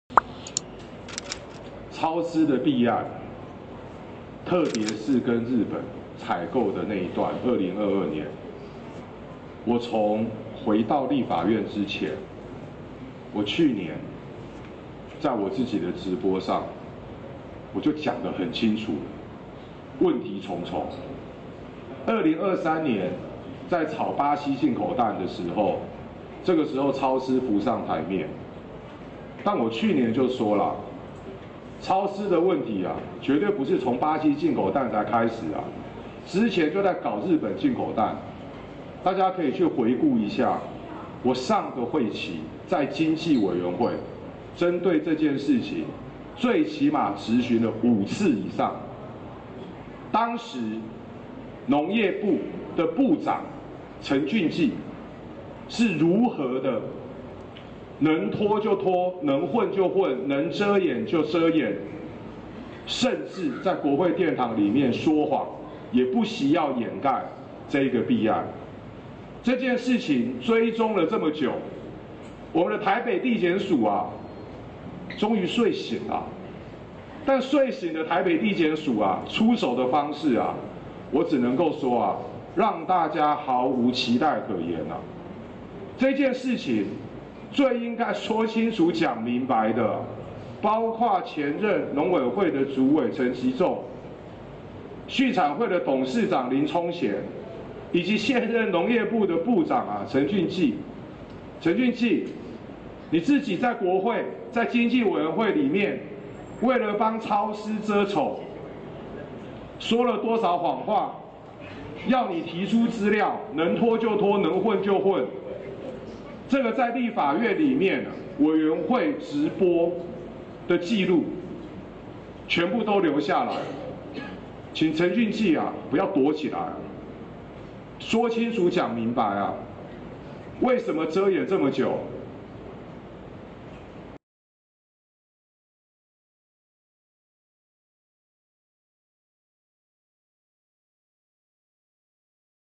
進口蛋涉弊！超思負責人200萬交保 黃國昌點名陳吉仲、林聰賢、陳駿季講明白｜新聞原味｜華視新聞 20240925
農業部、中央畜產會，去年專案補助貿易商，進口大量雞蛋，爆發超思公司無照偷跑，還有重複申請補助款弊端，遭到監察院糾正！北檢昨(24)日搜索約談農業部副司長陳中興，以及超思負責人秦語喬，總共9人，5人請回、4人分別因圖利和加重詐欺罪，分別以30萬到200萬元交保。